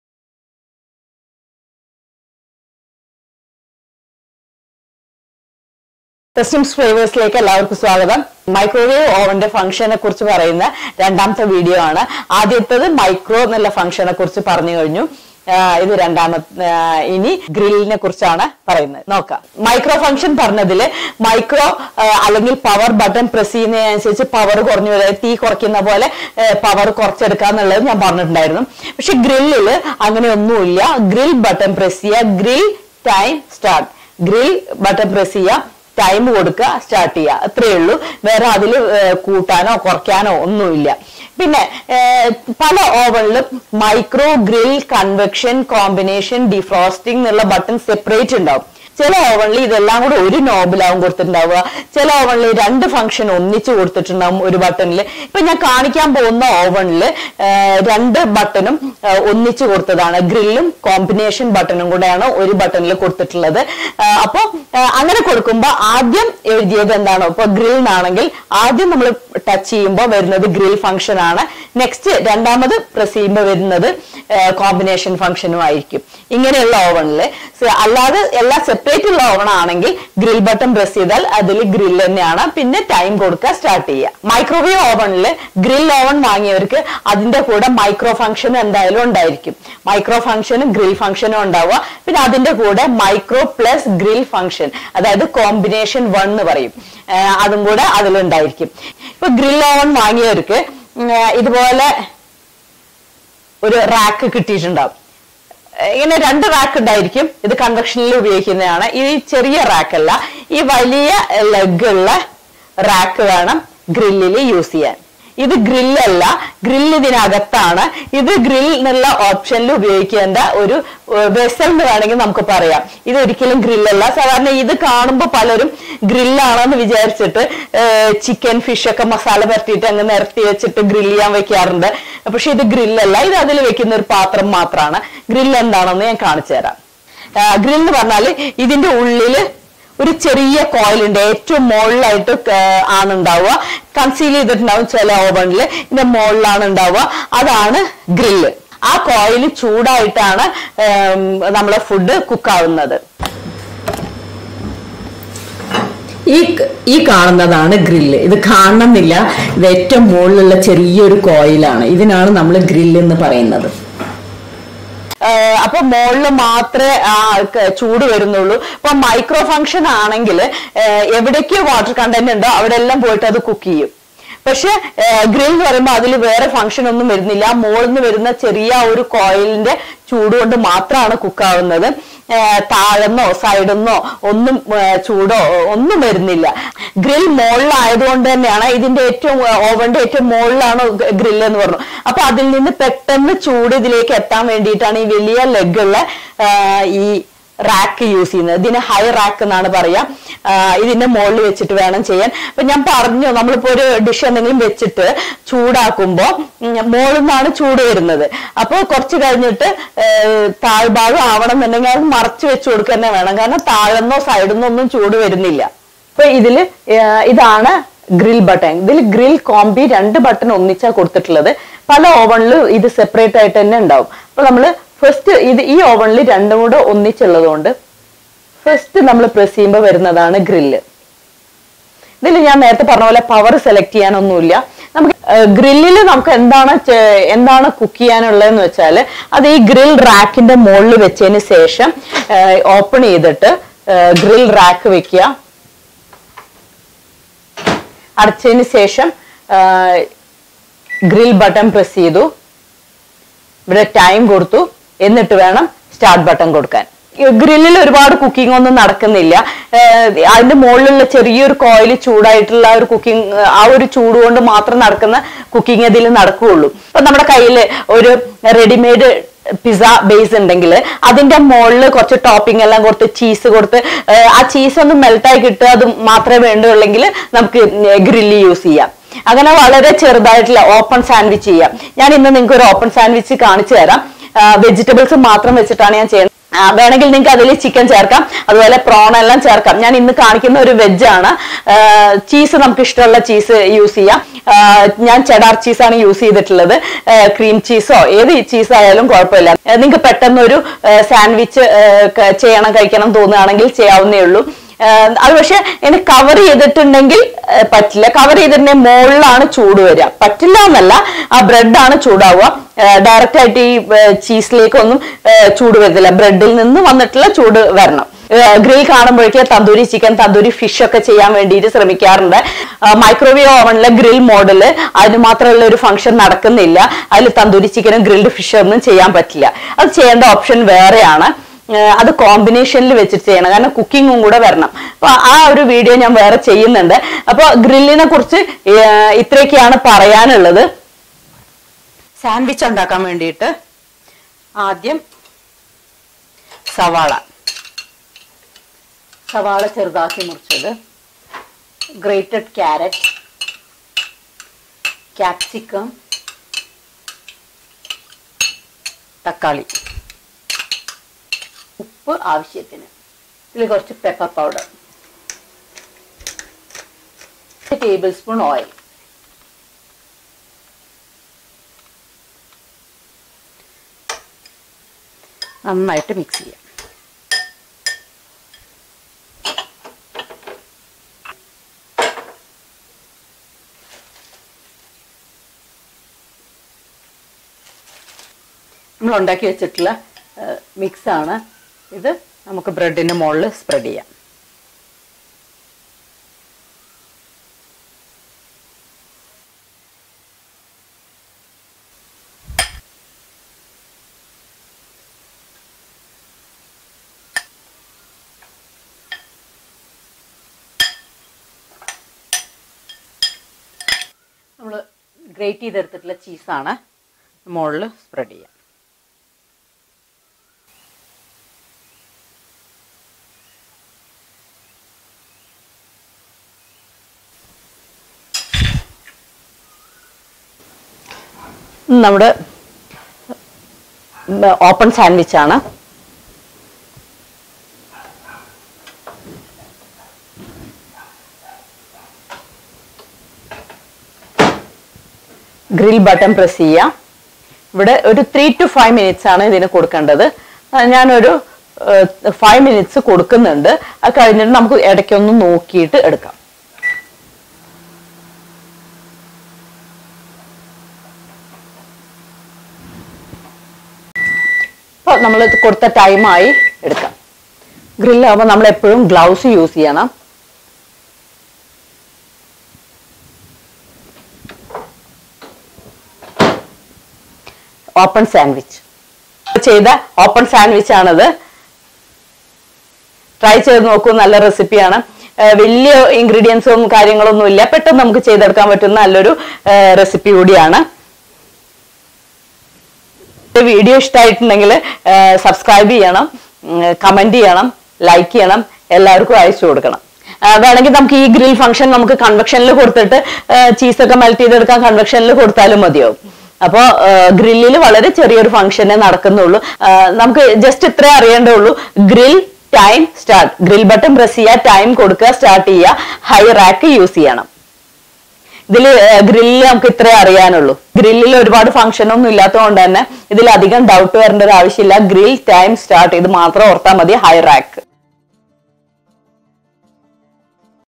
മൈക്രോവേവ് ഓവനിൽ എങ്ങിനെ ഗ്രിൽ ചെയ്യാം/Grill#open sandwich#Thesnim'sflavours#easy#microwaveoven
grilled chicken combination cooking

vessels use in Microwave oven

THESNIMSFLAVOURS#OPENSANDWITCH#GRILLSANDWITCH
bread slices
1 onion chopped
1small capsicum chopped
1tomato chopped
1carrot grated
cheddar cheese 1/2 cup
oil 1tbsp
pepper powder
salt
grill 3..5 mints